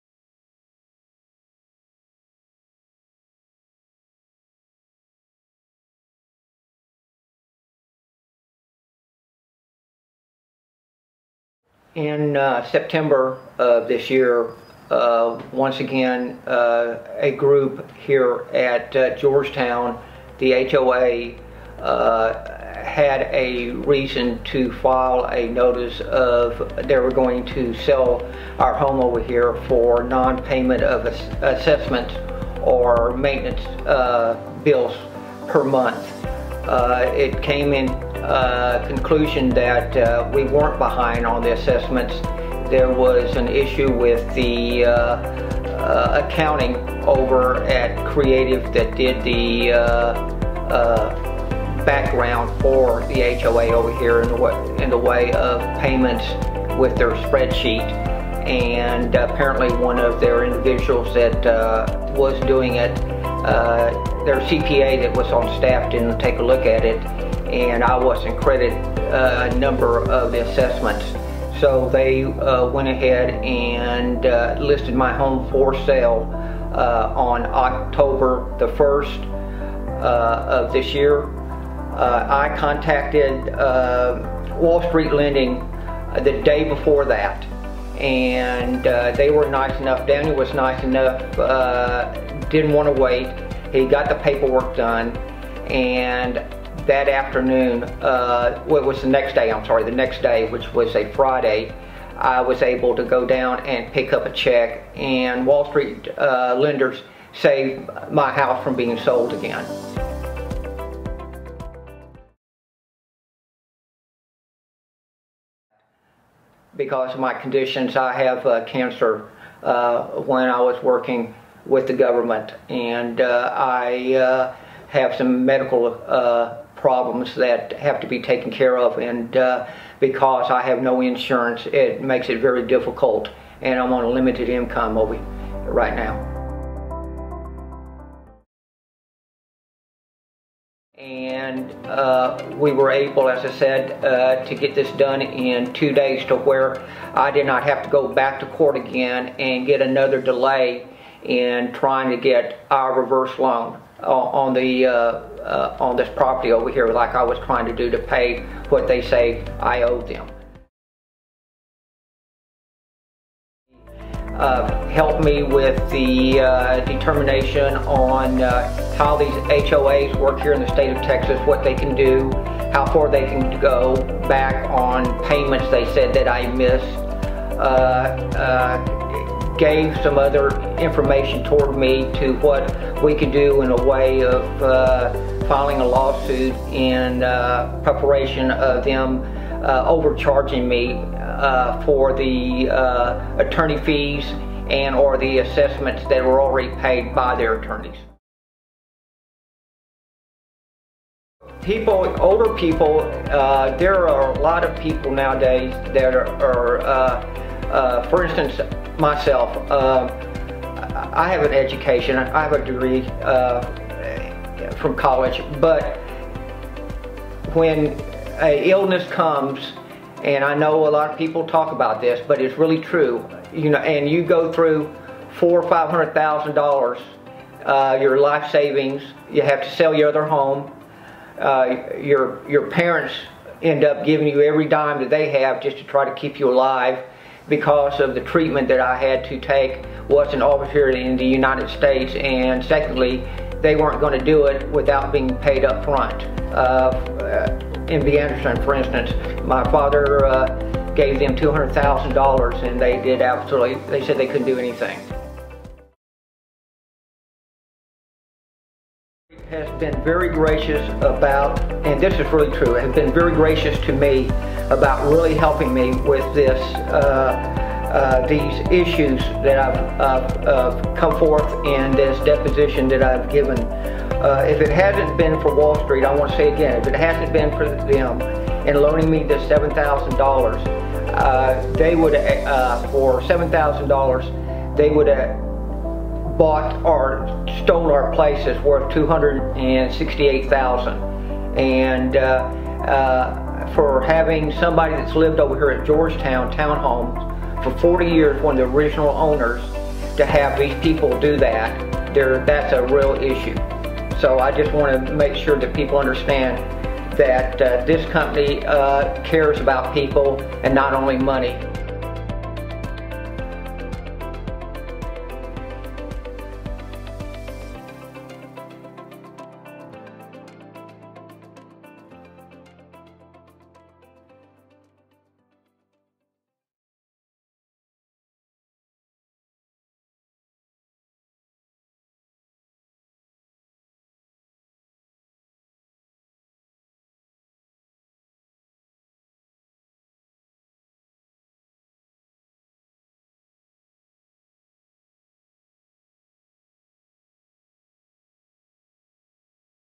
FBI Agent's Home Rescue | VinhXTruong Saves the Day!
In this captivating YouTube video, we bring you the touching story of a retired FBI agent on the verge of losing their home due to an HOA predicament. But, hold on tight, because our hero, VinhXTruong, AKA Captain Save A Home, emerges to rescue the day! Join us on this rollercoaster of emotions as we witness the challenges, the triumphs, and the unwavering commitment to helping homeowners in need. Get ready to be inspired by the power of compassion and real estate expertise. Don't miss out on this incredible journey! 🏠💪🌟